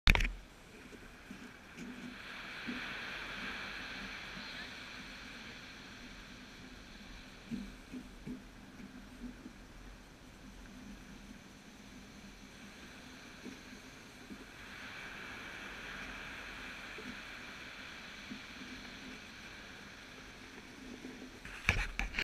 Sunset in Tenby, Wales
Just set up the camera on the sand. Tenby is a beautiful place. Go there if you can. Buccaneer Inn Pub has great food...just saying :)